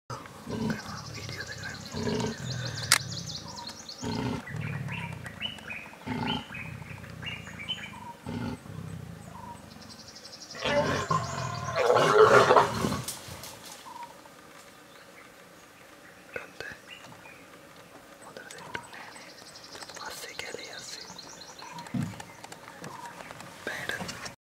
One of my most treasured sightings Sri Lankan Leopard (Panthera pardus kotiya) -Yala national pa.🔞🔞
One of my most treasured sightings - a pair of mating leopards. They mated in the bush all afternoon (we could hear them)🔞🔞🔞..
Sri Lankan Leopard (Panthera pardus kotiya) - Yala national park - Sri lanka